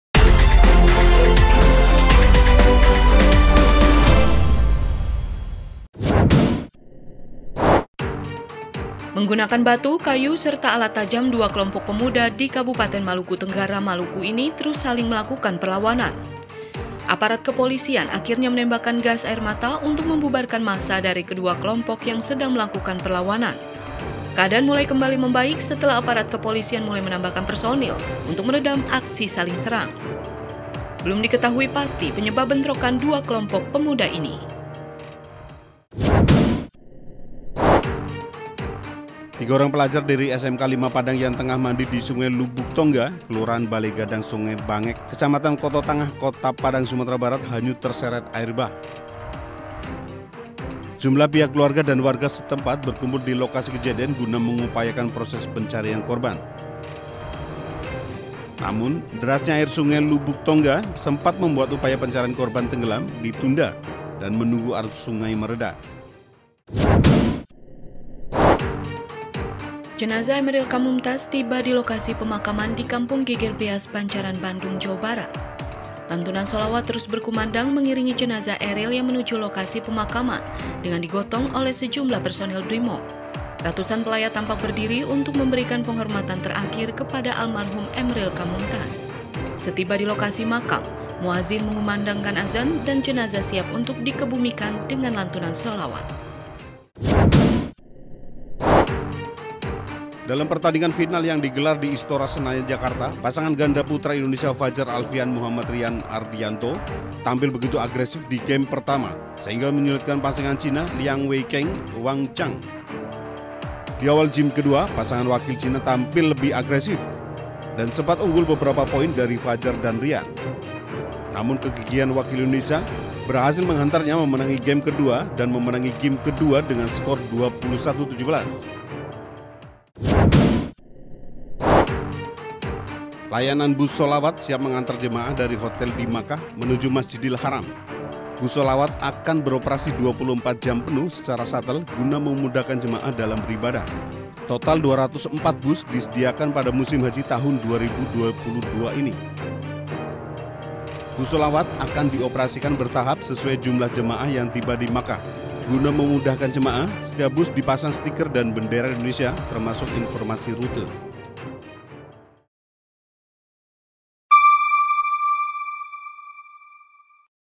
TOP 5 News: Dua Kelompok Pemuda di Maluku Terlibat Saling Serang, Tiga Pelajar Terseret Arus Sungai
Lima berita pilihan dalam Oke 5 News, Senin(13/6).

Berita pertama, Dua kelompok pemuda di Maluku Tenggara, Maluku, terlibat bentrok. Aksi saling serang, menggunakan batu dan kayu tersebut akhirnya dibubarkan Polisi dengan menggunakan tembakan gas air mata.

Berita kedua, Tiga orang pelajar dilaporkan hanyut terbawa arus sungai di Kota Padang, Sumatera Barat. Tim SAR Gabungan dikerahkan ke lokasi untuk melakukan pencarian Korban.

Berita ketiga, Jenazah Emmeril Kahn Muntadz, Putra sulung Gubernur Jawa Barat Ridwan Kamil, hari ini dimakamkan di Kampung Beas, Banjaran, Bandung, Jawa Barat. Ratusan pelayat memberikan penghormatan terakhir kepada Almarhum sambil melantunkan Salawat.

Berita keempat, Ganda Putra Indonesia, Fajar Alfia dan Muhammad Rian Ardianto menjuarai gelaran Indonesia Master 2022. Dalam pertandingan finalnya, Fajar dan Rian berhasil mengalahkan wakil Cina dua set langsung.

Berita kelima, Bus Salawat untuk para Jemaah Calon Haji di Makkah, Arab Saudi siap untuk beroperasi. Layanan Bus Salawat yang bersticker Bendera Indonesia ini, akan bersiaga 24 jam penuh, guna memudahkan Jemaah dalam beribadah.